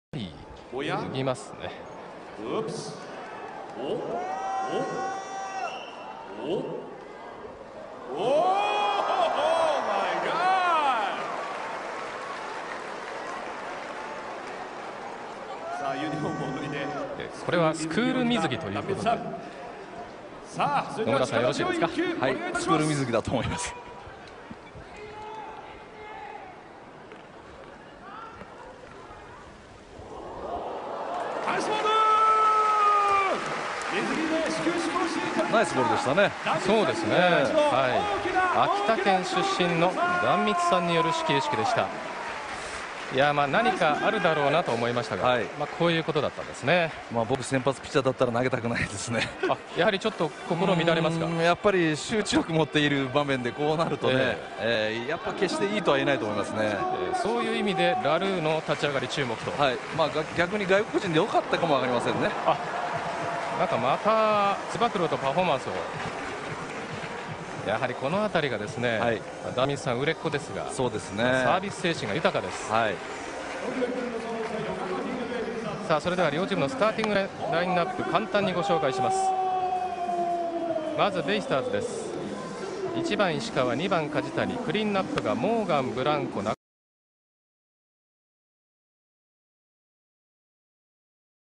Mitsu Dan Throwing First Pitch
Gravure idol Mitsu Dan trrows out the first pitch at a 2013 Tokyo Yakult Swallows - Yokohama Dena Baystars game.